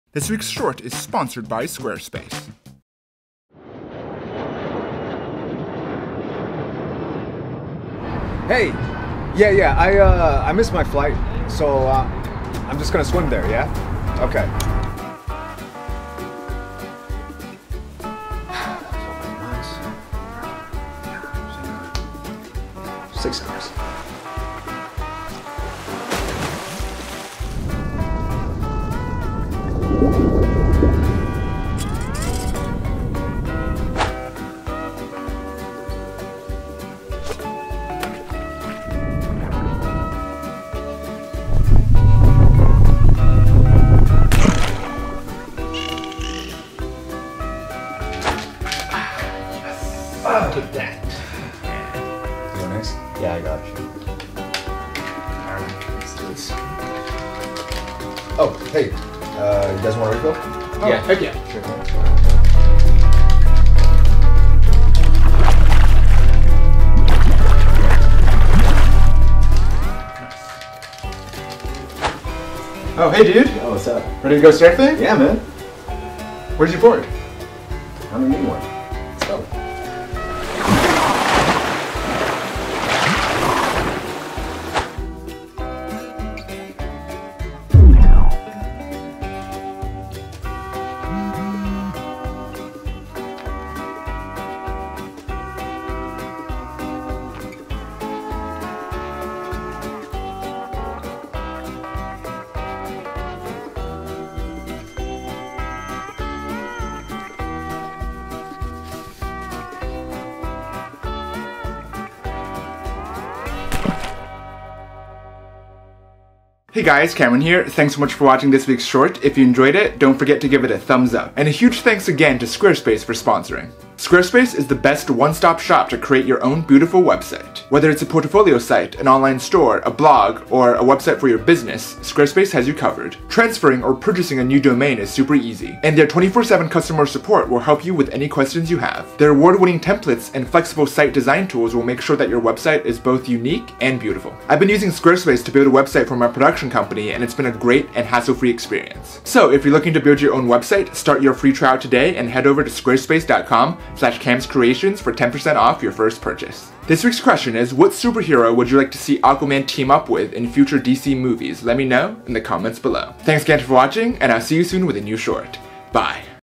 5 Everyday Uses of Atlantean Abilities (Aquaman) ft. Manny Shih
Thanks Manny for acting! Give him a follow

Credits:
Cast - Manny Shih Cameron Sun, Daichi Sakane
Director, editor, VFX artist & sound designer - Cameron Sun
Director of photography - Daichi Sakane